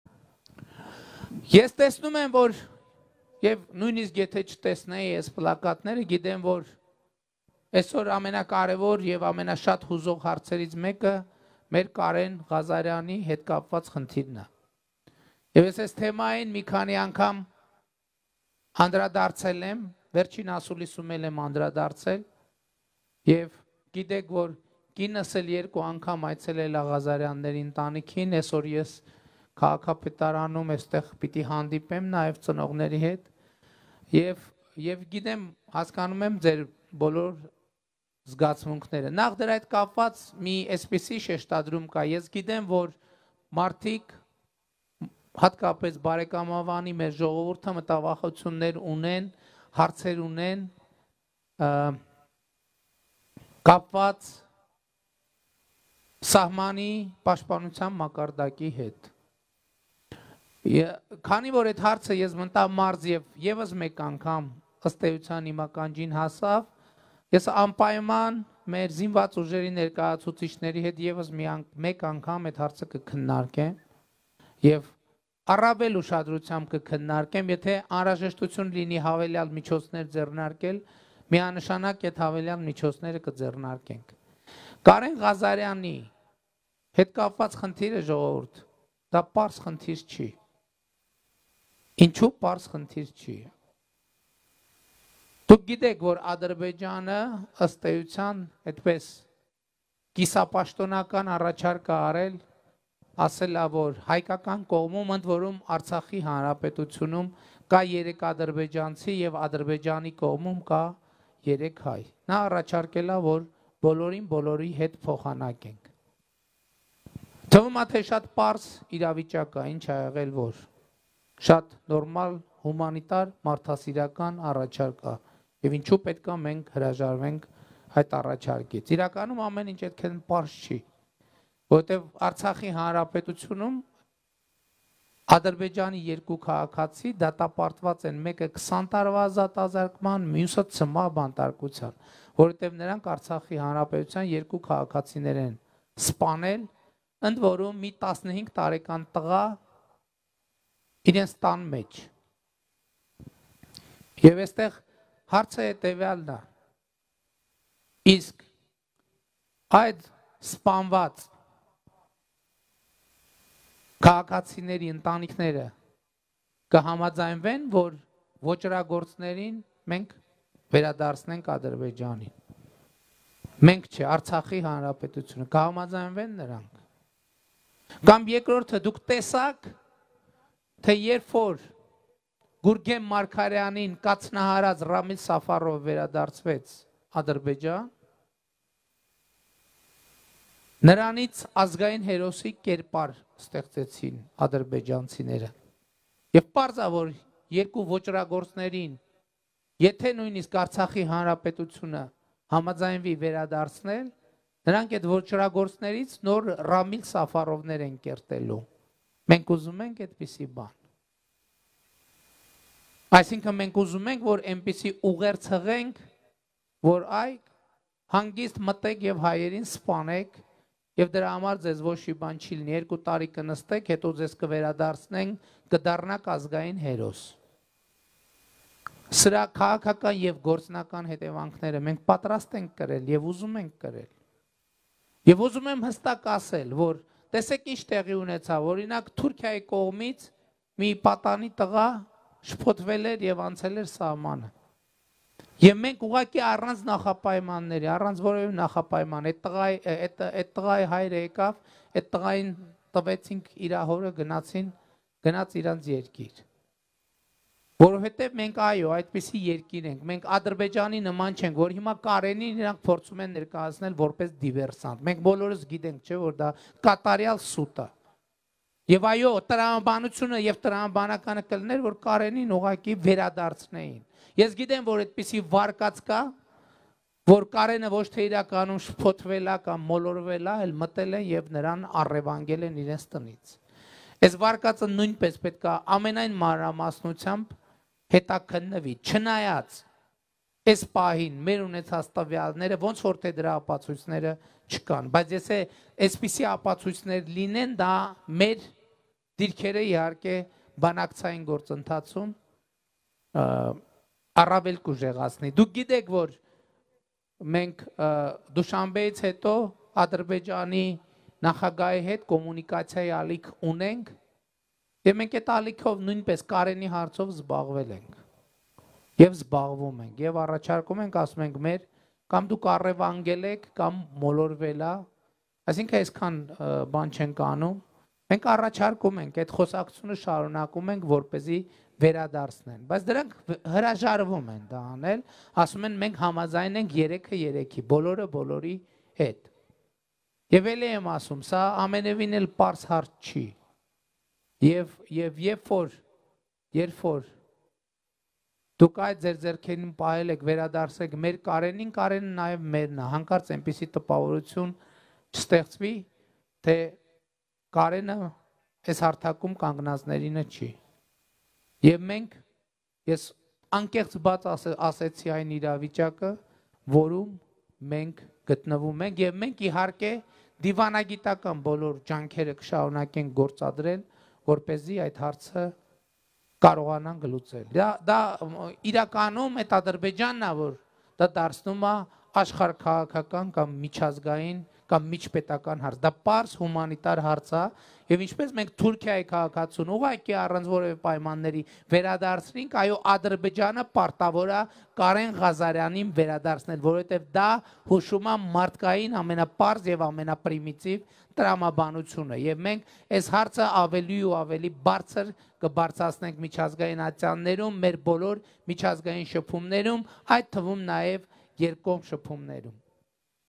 Ադրբեջանը պարտավոր է Կարեն Ղազարյանին վերադարձնել. Նիկոլ Փաշինյան
FACTOR TV - это информационный канал, вещающий 24 часа в сутки.Экономическая аналитика и интервью с влиятельнейшими персонами.

➤Factor TV в OK.ru news